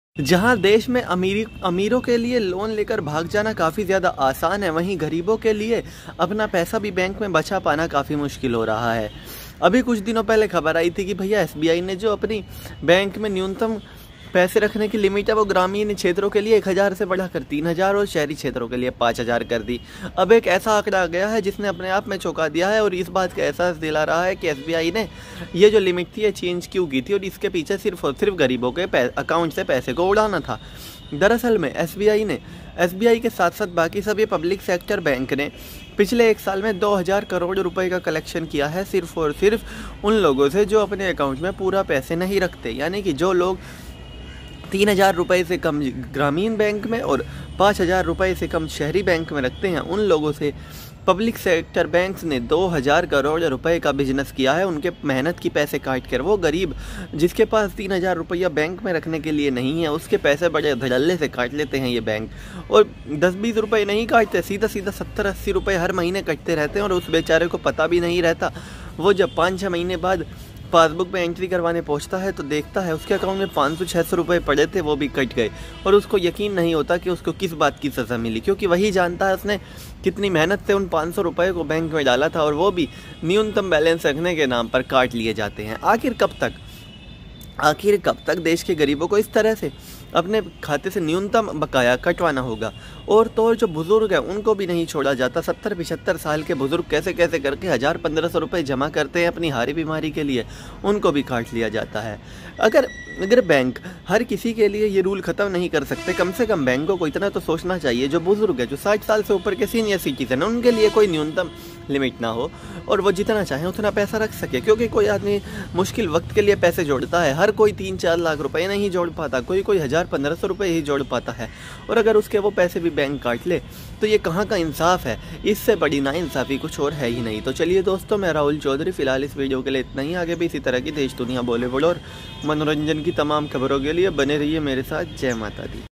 Public sector banks earned 1996 crores in last one year from minimum balance deduction
Public sectors banks collected rs.1996 by deducting the penalty against not maintaining minimum balance.